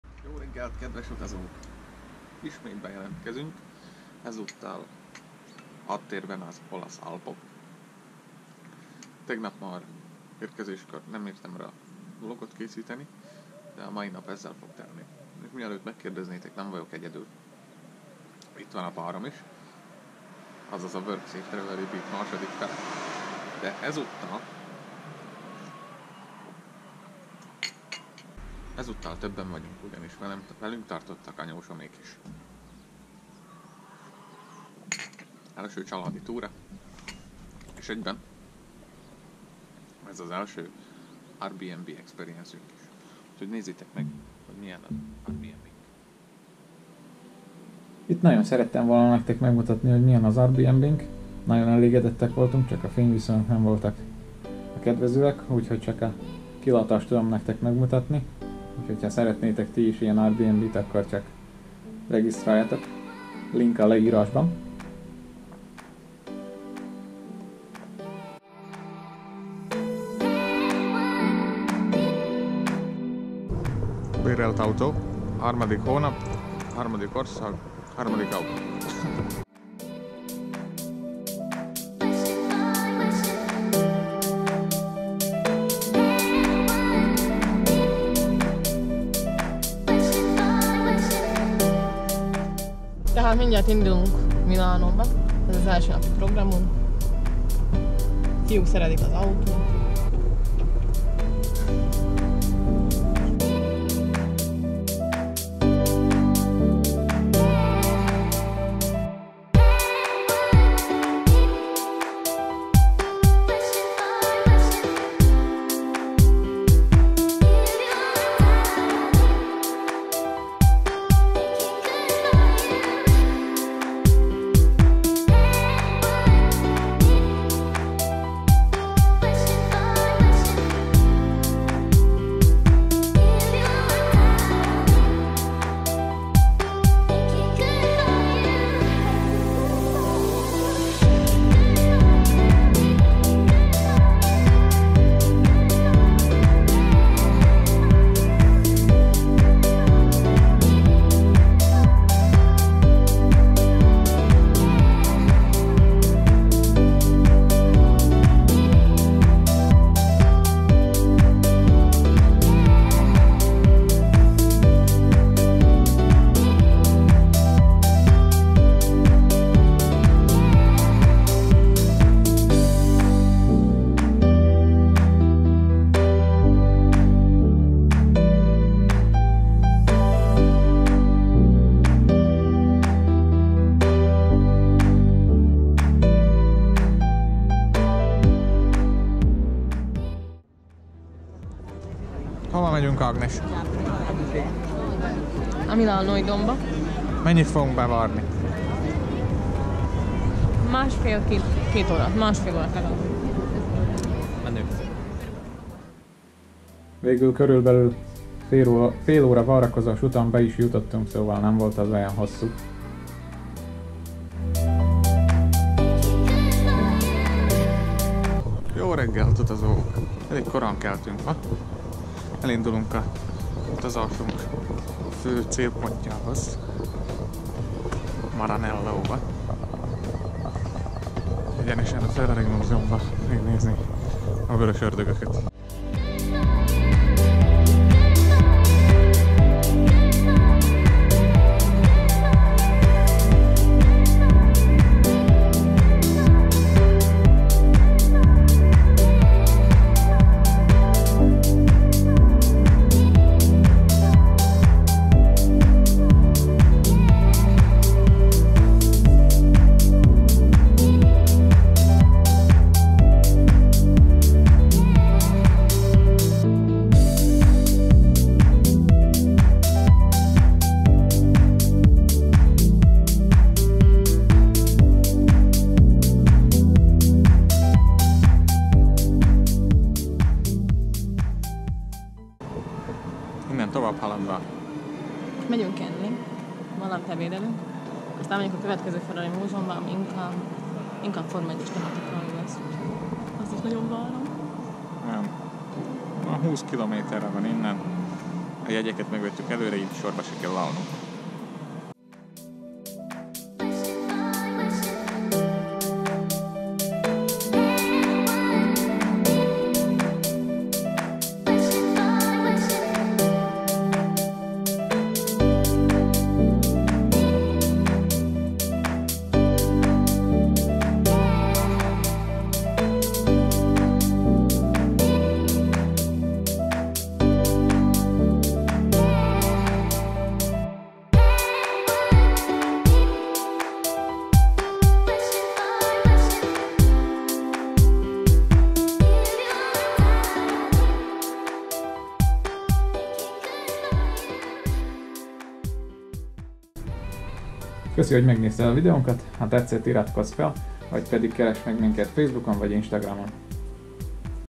MILÁNÓ | MARANELLÓ | MODENA - Ferrari múzeumok | VLOG #10 | (SK sub.)
Hosszú hétvégét töltöttünk Észak-Olaszországban. Ezúttal egy családi kirándulás keretében. Ha tetszik a videó ne felejtsetek el feliratkozni. :)

Airbnb-re itt tudtok regisztrálni, ezzel minket is támogattok és nektek is jár a kedvezmény :)

Gimbal : Feyiutech G5